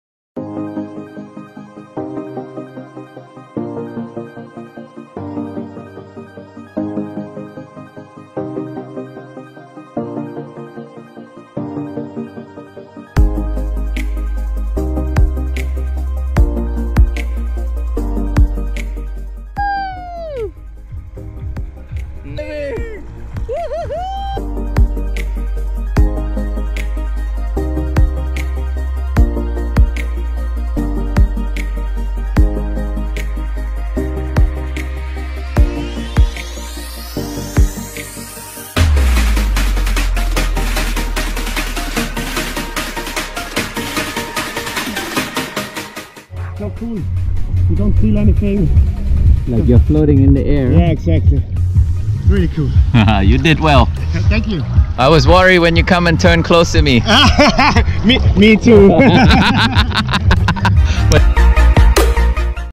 E-foiling on Koh Phangan 20220913
Haad Rin can be a fun place for many beachgoers and also water sports enthusiasts. Electric foiling has become popular this year and with proper instruction, one can be gliding above the water in no time.